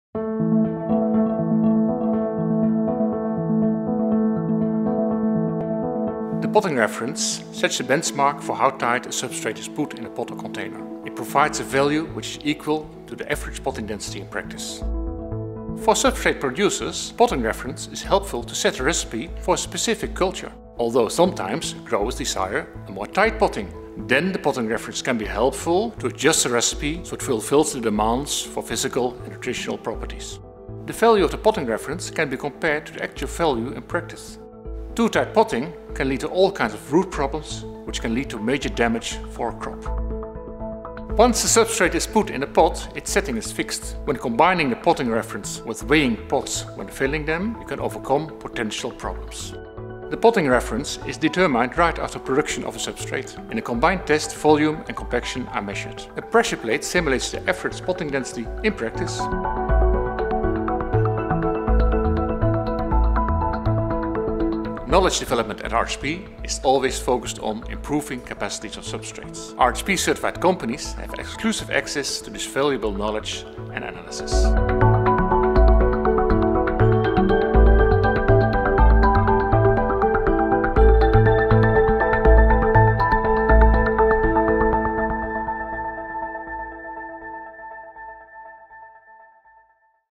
RHP explanatory video - Potting reference
The potting reference developed by RHP provides insight in the density of the soil through potting. This helps at the choice of a substrate that fits the potting behaviour and the way of cultivation and in this way avoids root problems and culture damage as a result of potting too sturdy.